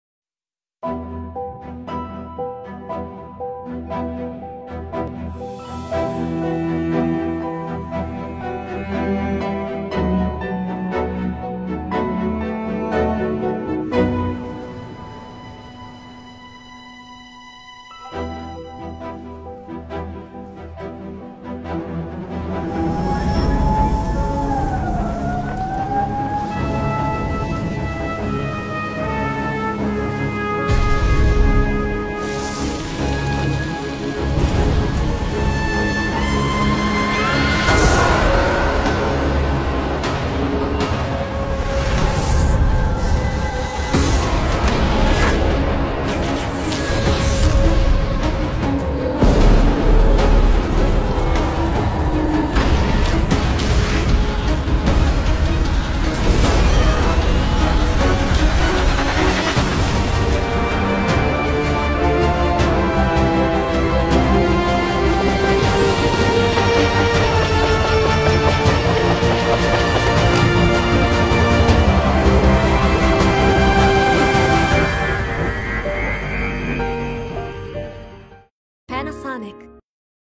VariCam35 4K HDR TV Commerical Hollywood to Your Home
Brand new video shot on VariCam 35 and produced exclusively for Panasonic 4K Pro HDR OLED TVs.